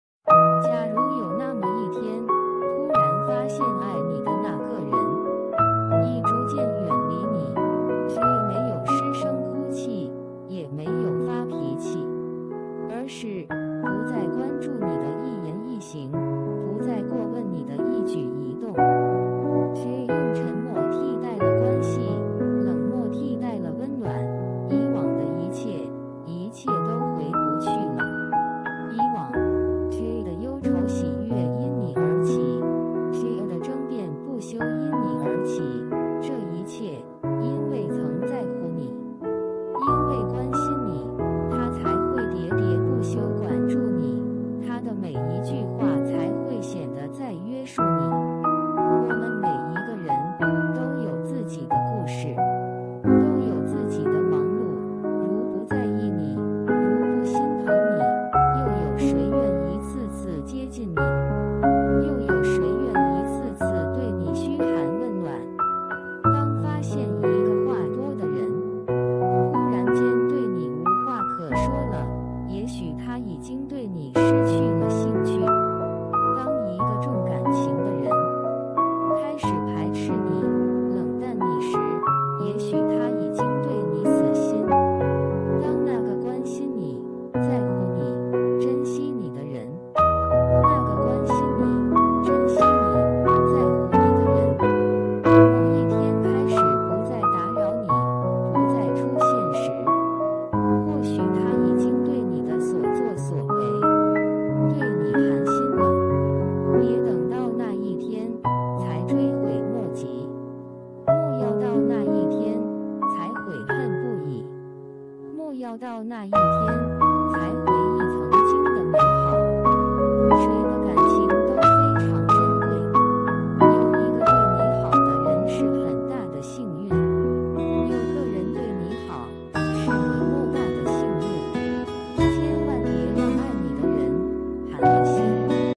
別讓愛你的人，寒了心...
假如有那麼一天，突然發現愛你的那個人，
已逐漸遠離你，Ta沒有失聲哭泣，也沒有發脾氣。
而是，不再關注你的一言一行，不再過問你的一舉一動。
Ta用沉默替代了關系，冷漠替代了溫暖；以往的一切，一切都回不去了。